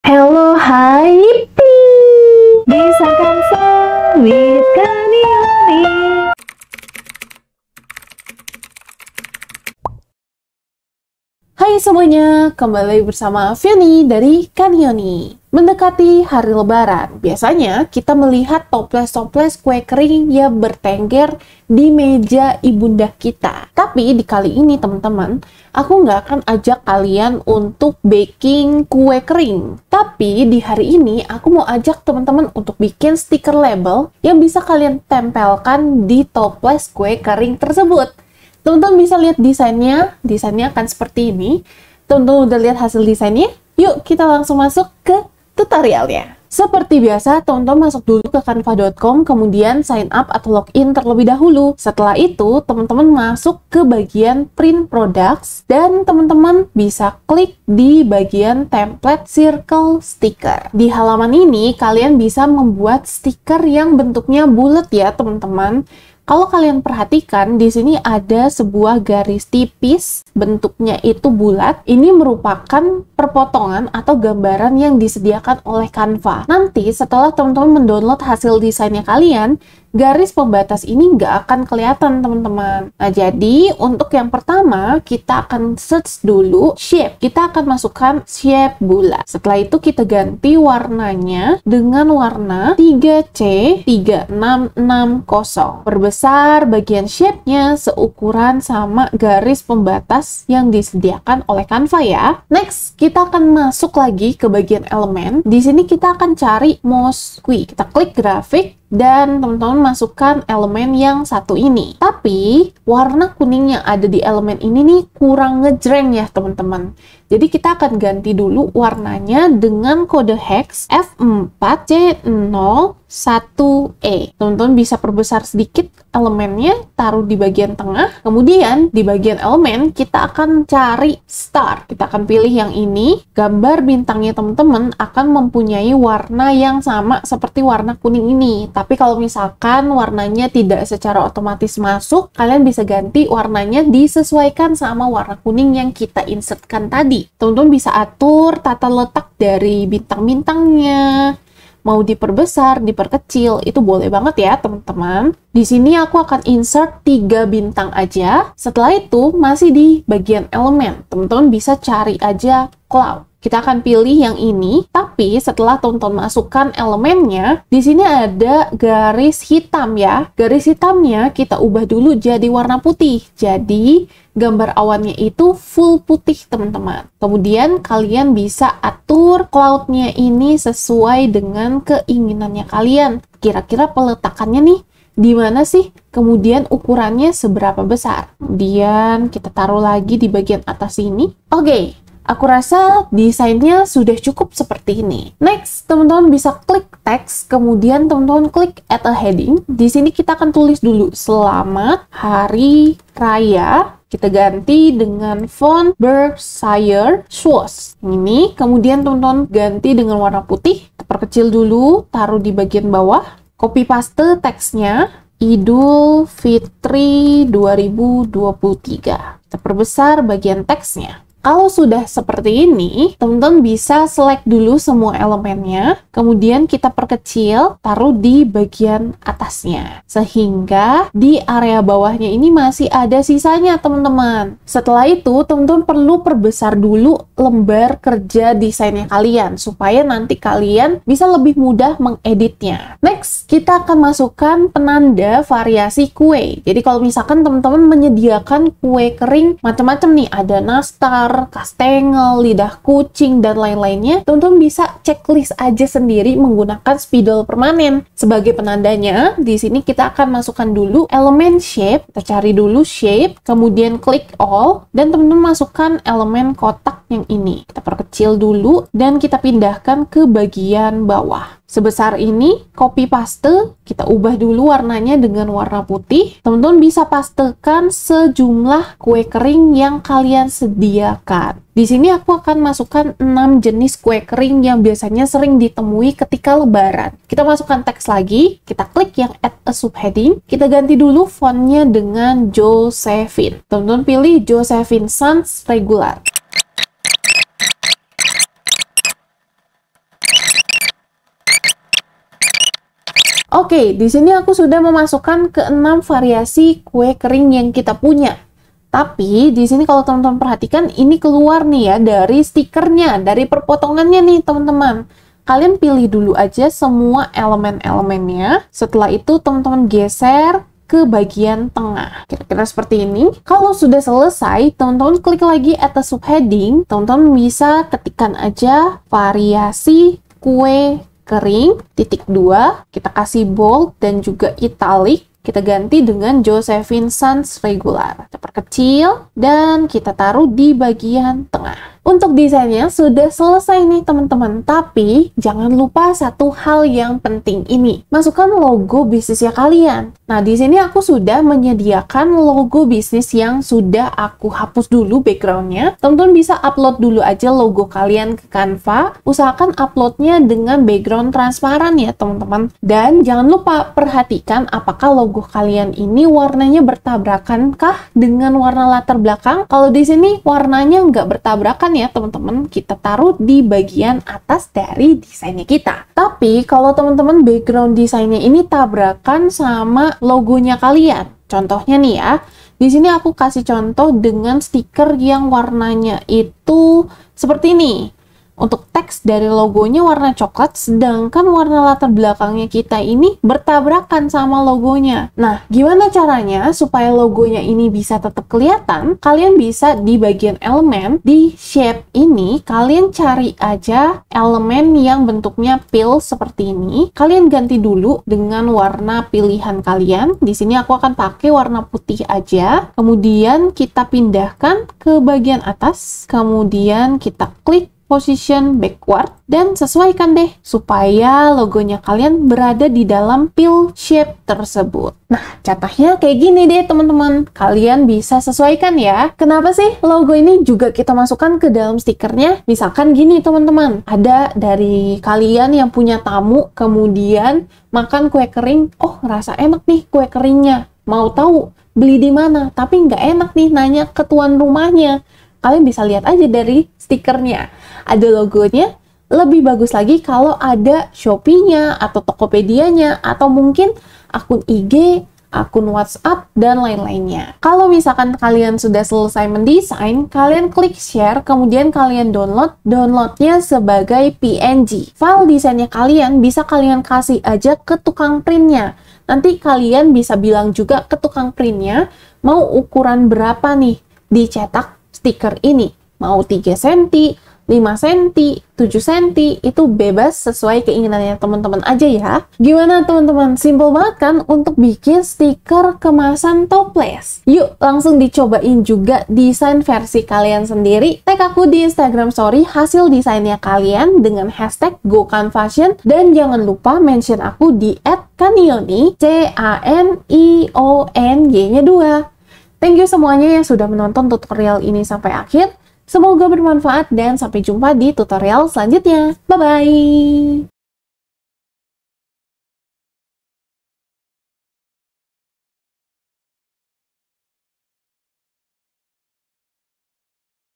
Tutorial Stiker Toples Kue Kering di Canva FREE
Hai-halo dan annyeong!
Selamat datang di Youtube Canionyy yeaayy~

Menjelang waktu lebaran, mungkin ada di antara teman-teman yang mau jualan kue kering toples. Nah, supaya kue keringmu lebih berkarakter dan punya ciri khas, teman-teman bisa tempelkan saja stiker sederhana seperti ini. Jangan lupa tag aku di IG atau tiktok @canionyy ya.

Mau request tutorial? Boleh banget dong! share aja request kamu di kolom komentarnya ya ^^

Mau langganan Canva PRO? atau mau dapet ebook gratis?

Semoga kalian suka sama tutorialnyaaa~